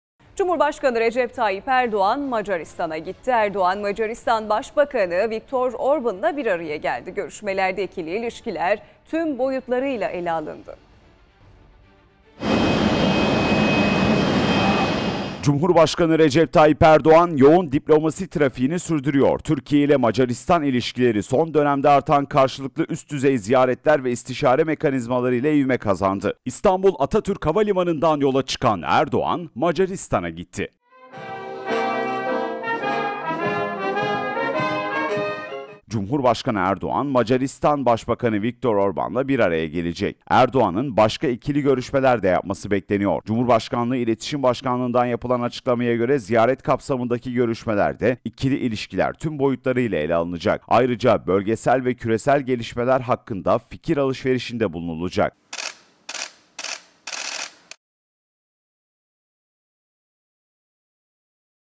Cumhurbaşkanı Erdoğan Macaristan'da! Vize Serbestisi ve AB Üyeliğinde Yeni İvme - TGRT Haber
Cumhurbaşkanı Recep Tayyip Erdoğan, Macaristan'a gitti. Erdoğan, Macaristan Başbakanı Viktor Orban ile bir araya geldi. Görüşmelerde ikili ilişkiler tüm boyutlarıyla ele alındı.